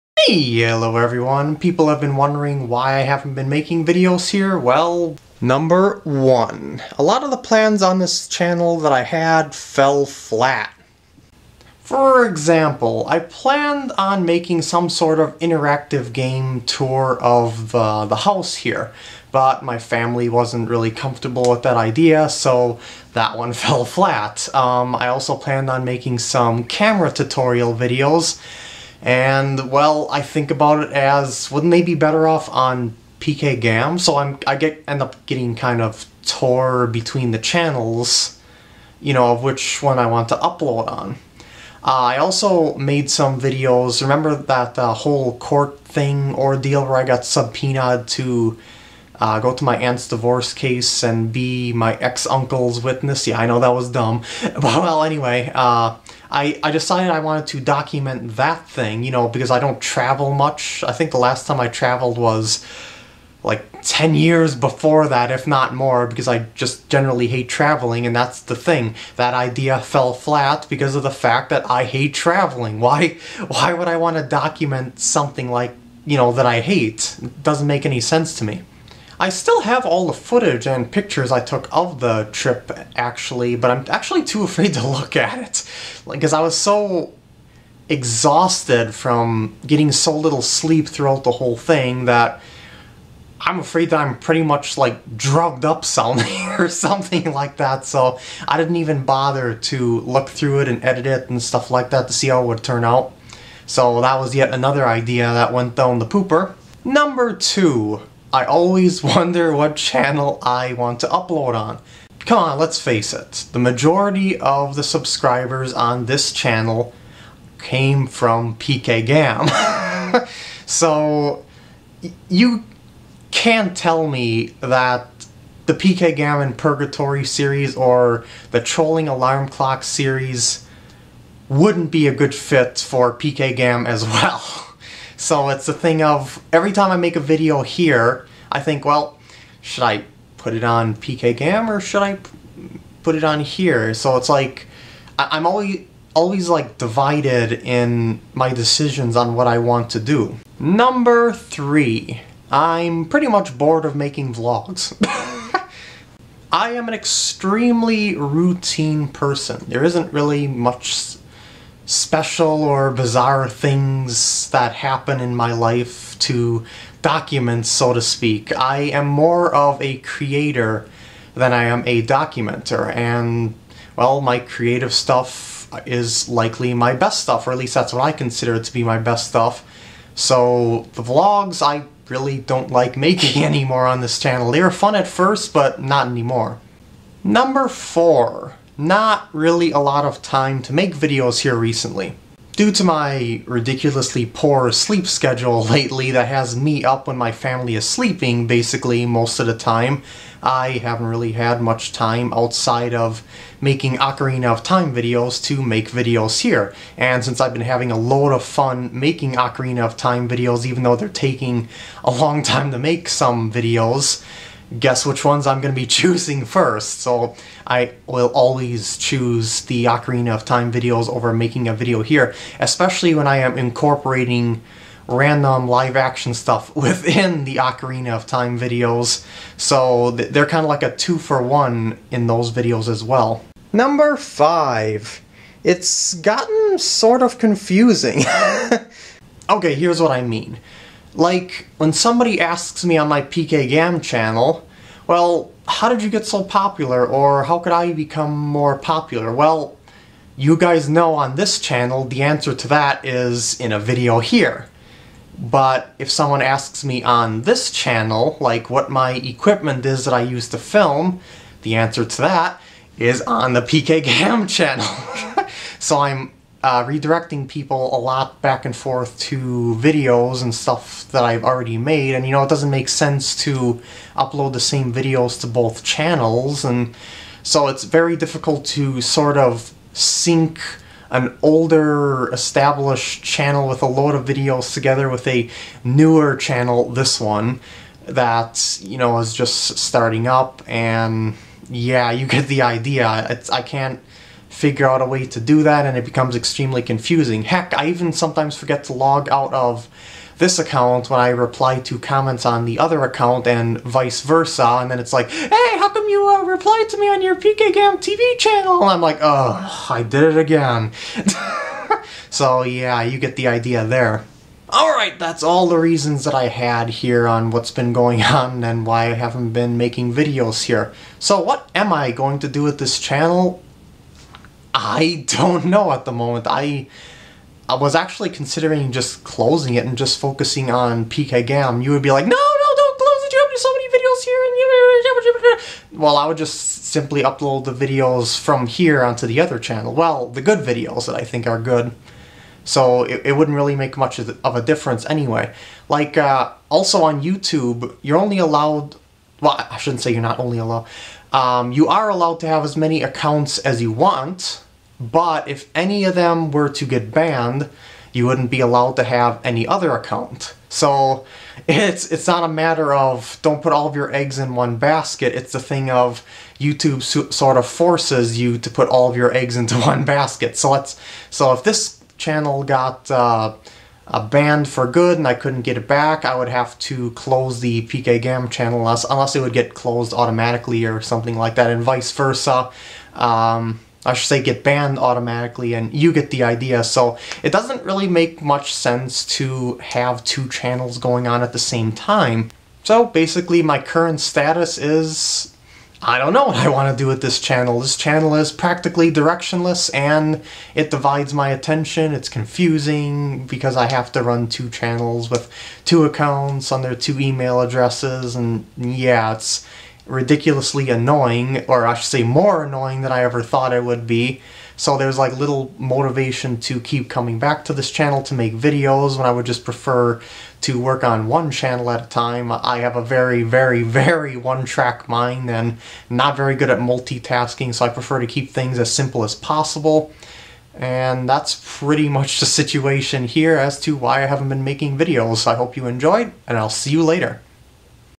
Vlog: Why haven't I been making videos here? (October 18, 2011)
(Re-upload to test something, I'll explain later if it works. The original upload date is in the title anyway. Edit: Nope, what I was going for didn't work. Oy!)

Ohh... I felt really awkward making this video given that I have been posting on this channel for close to a year now and am giving this news out. But, this is how I feel at the moment about it. Clearly I'm just not into making videos here given how few videos I've made here as opposed to the many I churn out on PkGam. XD

I'd rather be honest than keep you in the dark and string you along. I really don't know what I want to do with this channel and when I do, I question if I should put the video here or on PkGam.

I could just leave this be and focus on PkGam as I have been, but there's no point in having this one then since having multiple accounts on Youtube is effectively useless.

This channel will naturally get comments I'll reply to, making me have to go back here then anyway, even if I post nothing more on this channel. So, I'll end up being active on here regardless.

Well, uhh... sorry to give you bad news about this channel, if it really can be considered "bad" since I'll still be making videos elsewhere anyway. :P